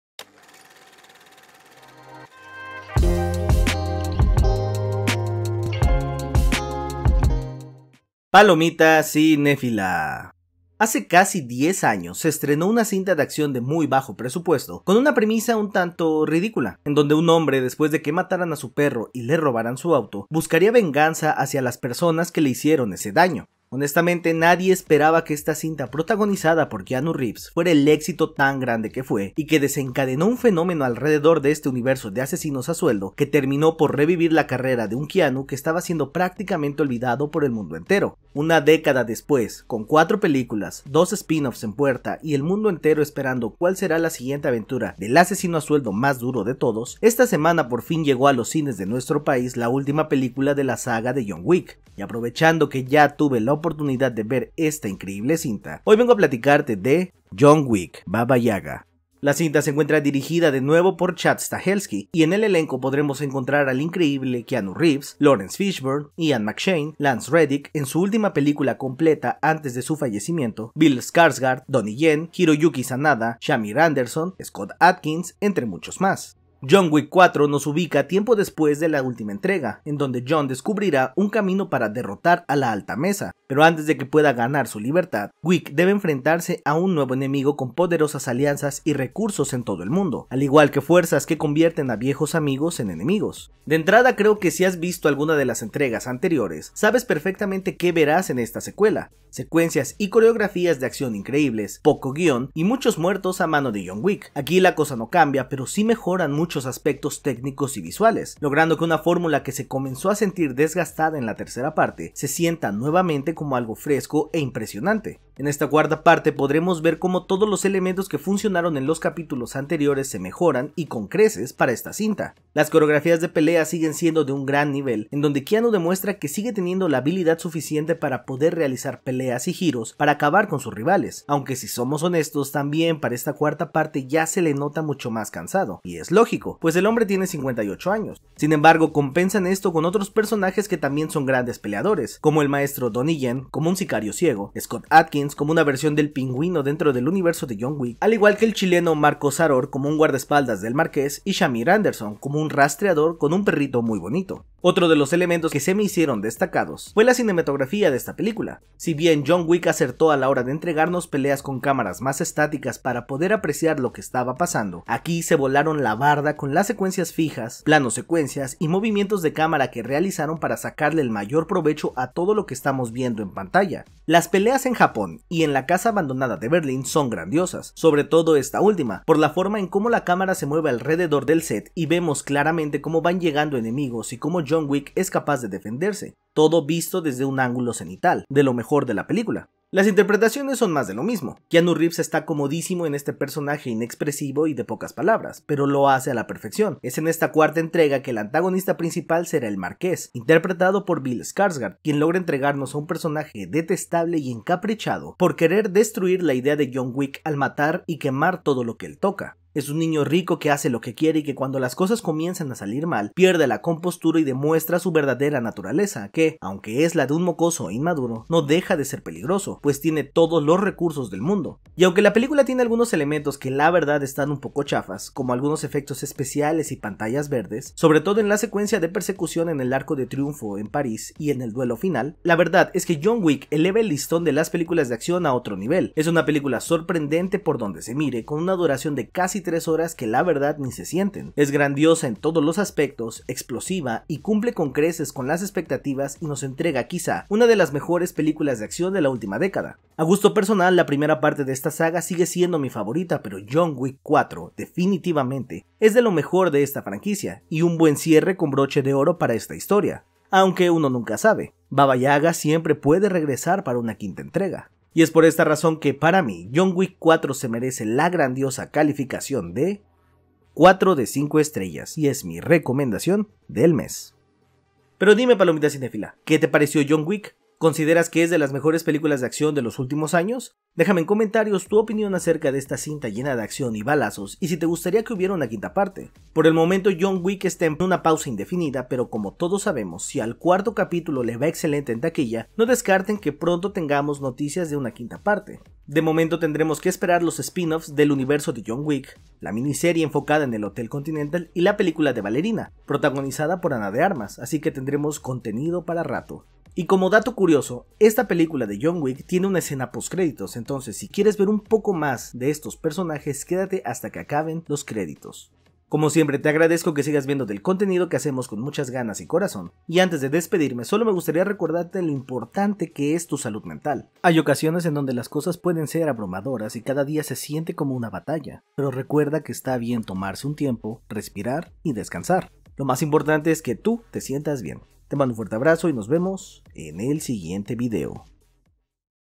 JOHN WICK REGRESA MÁS FUERTE QUE NUNCA
¡Palomita cinéfila! Por fin llegó a los cines de nuestro país la última entrega de nuestro asesino a sueldo favorito:

JOHN WICK 4: BABA YAGA

Y honestamente, esta nueva entrega de John Wick, eleva el listón muy alto para las películas de acción.